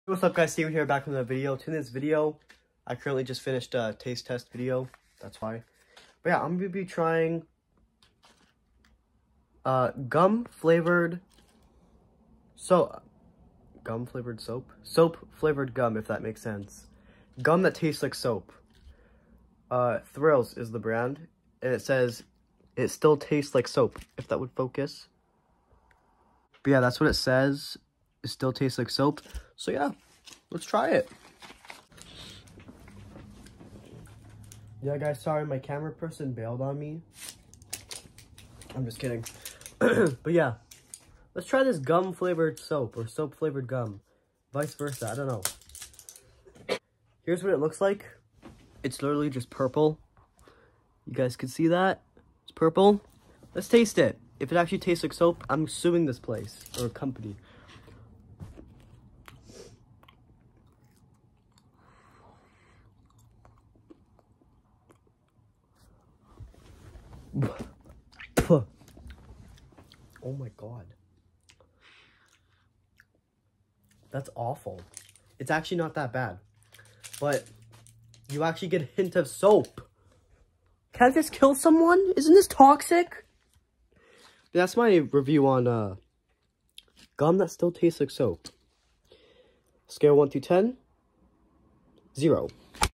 Trying Gum That Tastes Like Soap…
Hey guys, in this video I tried gum that tastes like soap… Nuff said